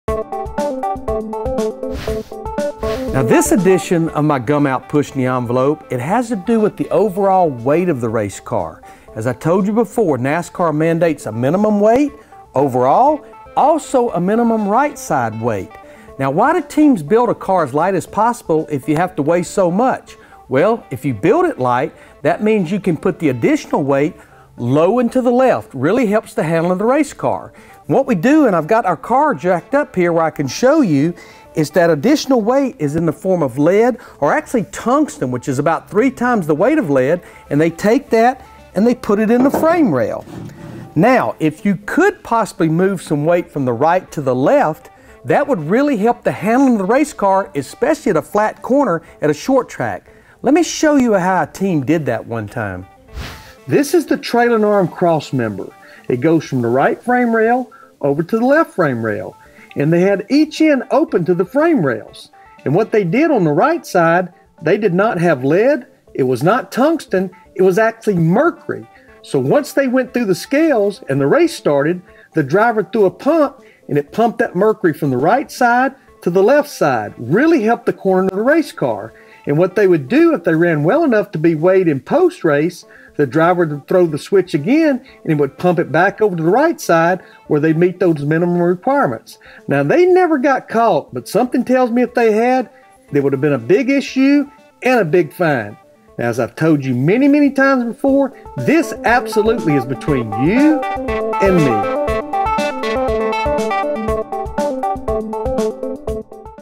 Larry Mac's Pushing The Envelope - Moving Mercury
There's lead, there's tungsten and there's a whole another substance called mercury, which we all know from thermometers.  See if you can figure out where this is going..